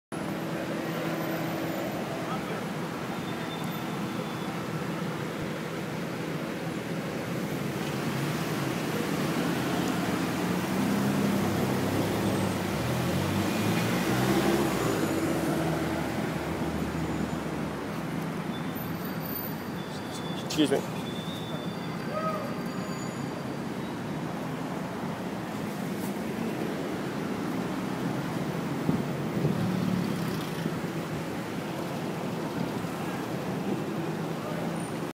Taxi Cab Fender Bender Chicago July 22, 2010
A video of the scene after a taxi cab tried to creep up the left hand side of the one way street's left hand lane. I took a left to go into Rock 'N' Roll McDonald's and he was in the left shoulder trying to drive up the left shoulder.